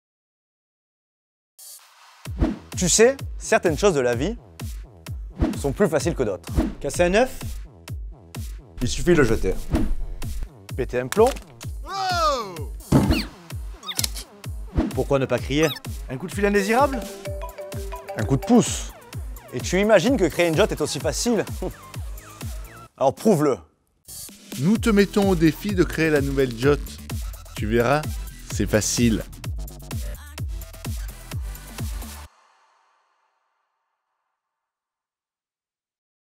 JOTTChallenge
Nico & Mathieu, les fondateurs de JOTT ont un message pour vous les JOTTeurs !
Alors, prêt à relever le défi ?